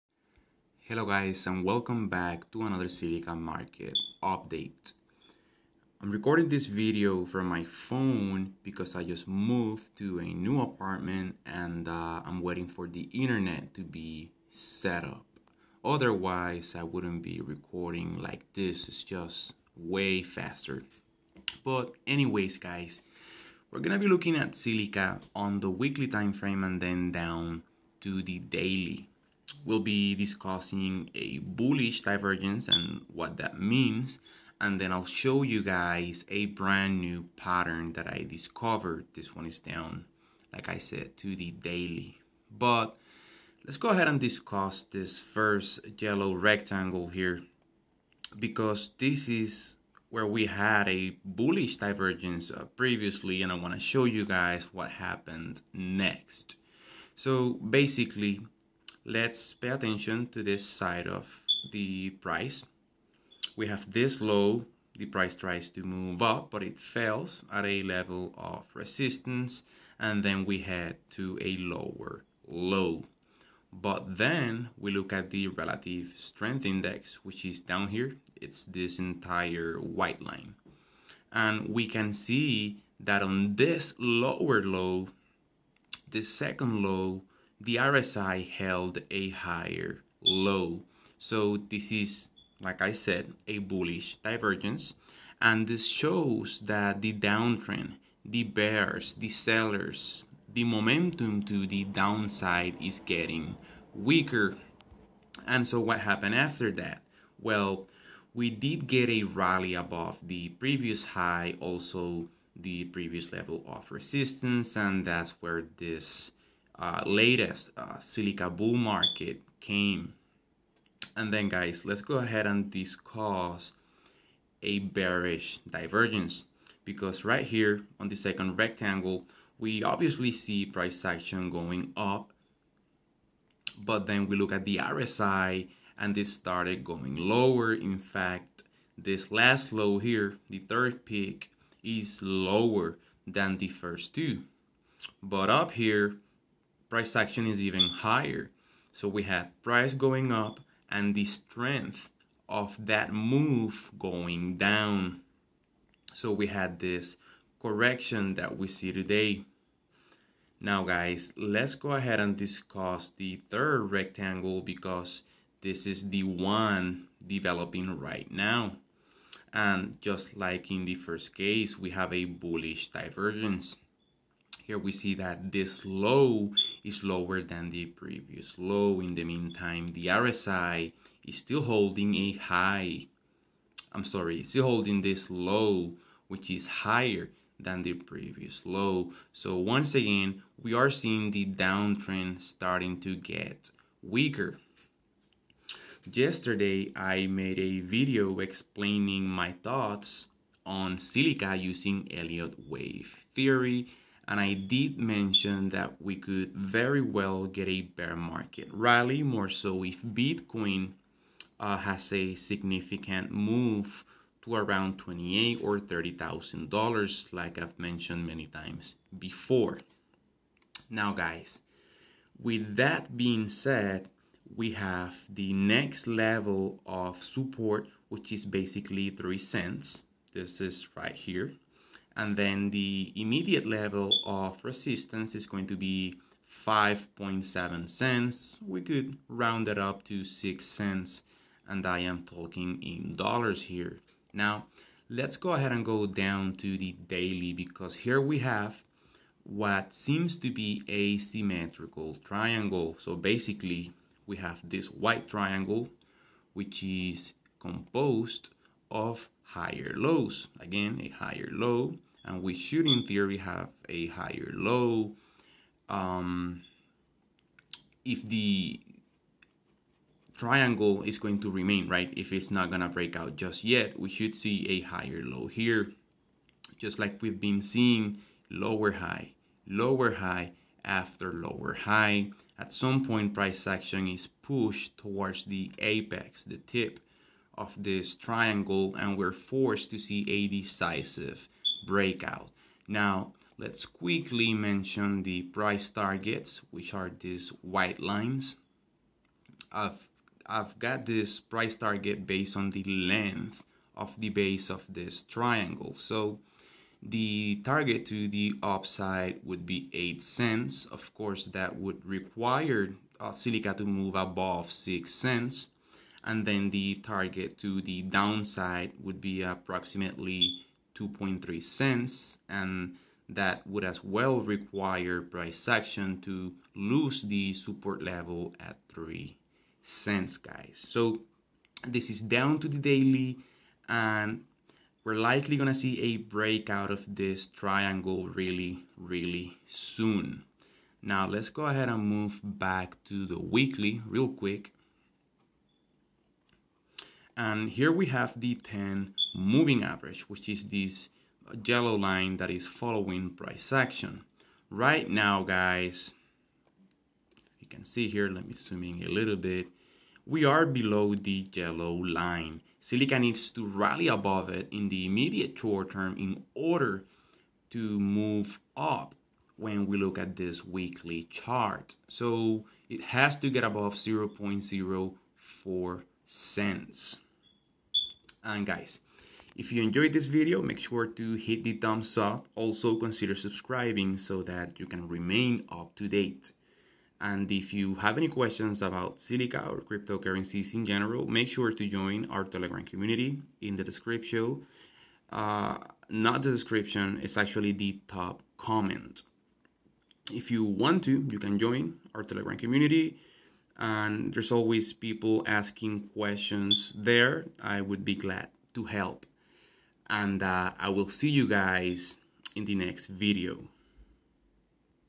Zilliqa Price Prediction - Get Ready! | Huge Move Soon
Zilliqa price prediction and chart analysis.